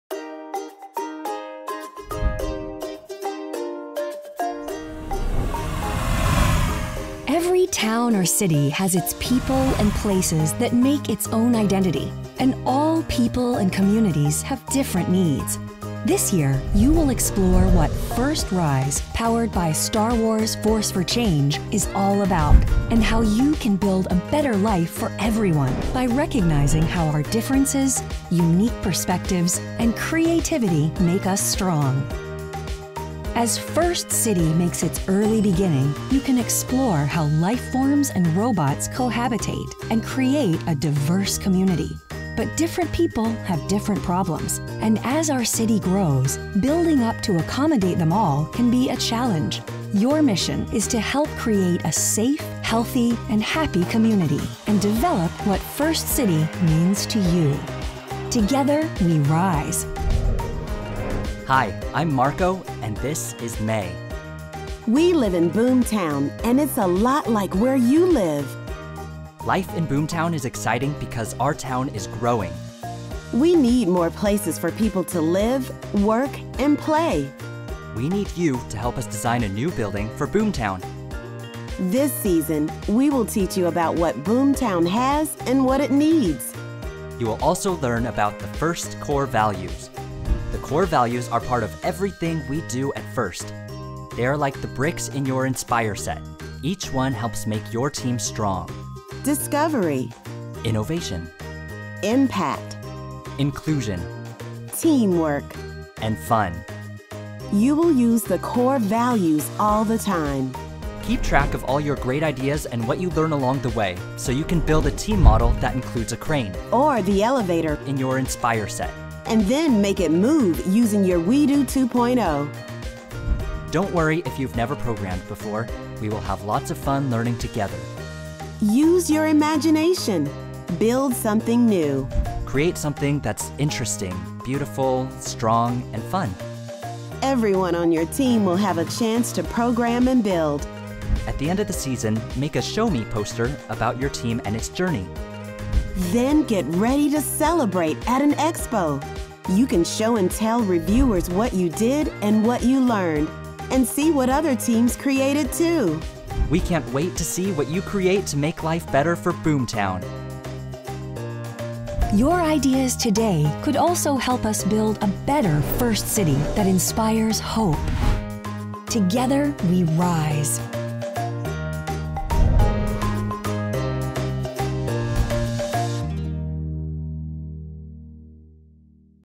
BOOMTOWN BUILD Kickoff Video
During BOOMTOWN BUILD, teams will explore what FIRST RISE powered by Star Wars: Force for Change is all about and how you can build a better life for everyone by recognizing how our differences, unique perspectives, and creativity make us strong.